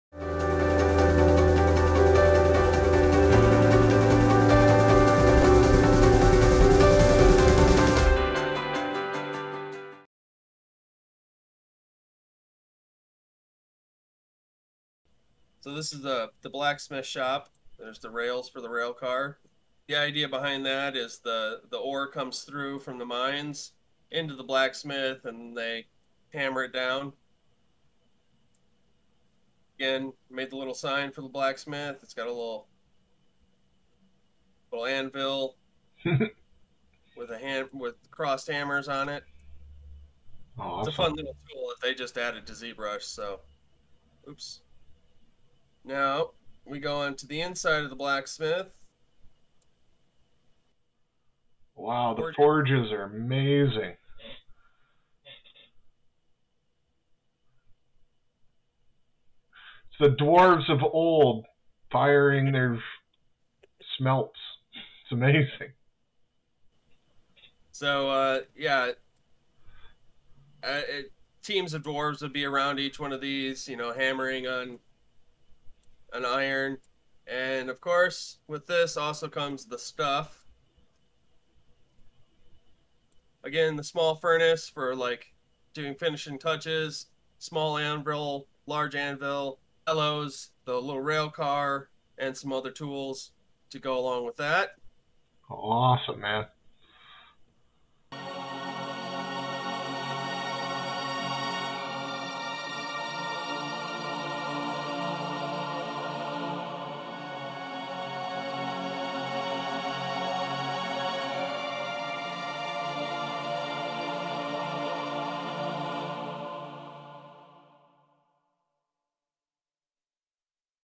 Dwarven Blacksmith
Short video interview with Dustin LeBlanc from Proxy Plus Minis showcasing the Dwarven Blacksmith from the September release from Imagin3Designs.